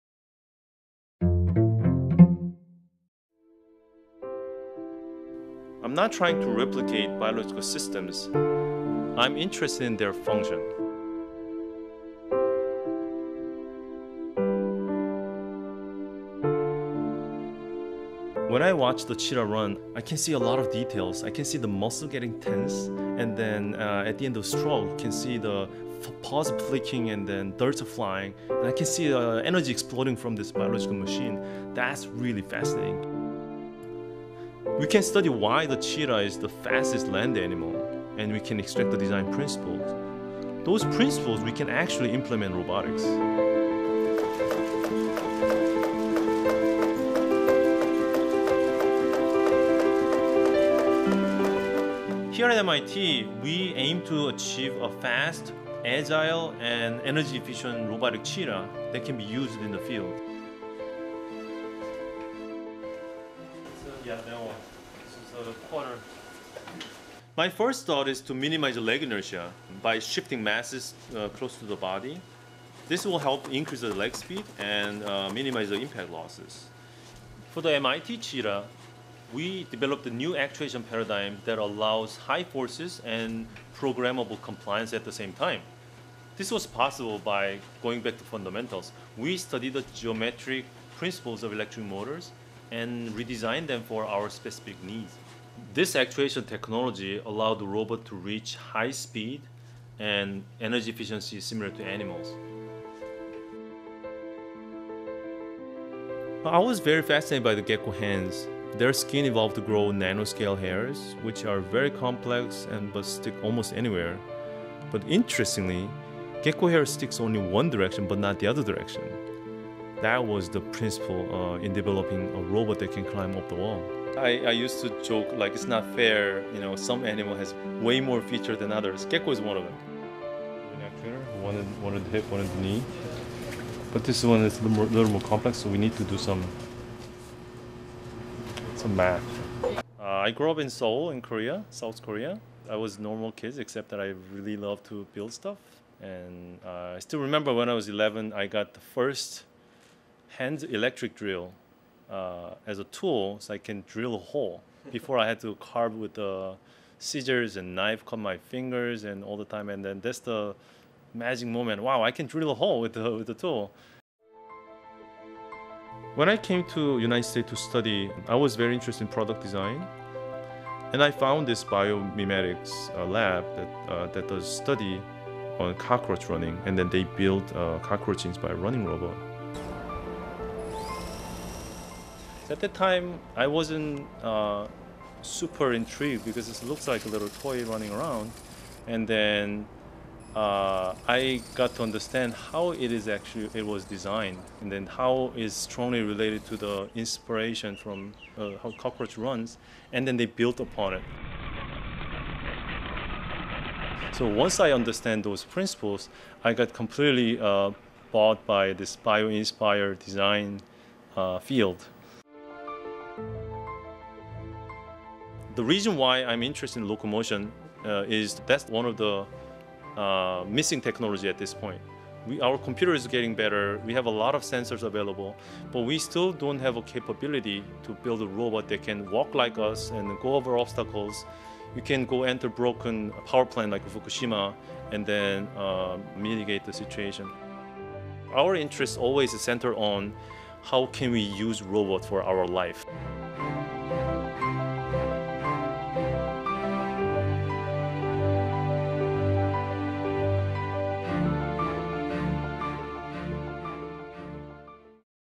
Sangbae Kim: Inspired by Nature
MIT Assistant Professor of Mechanical Engineering Sangbae Kim describes his cutting-edge research in the area of biomimetics, the study of biological systems as models for the design and engineering of robots. Professor Kim's current project -- a robot inspired by the special abilities of the cheetah -- runs at high speeds with an energy efficiency similar to animals.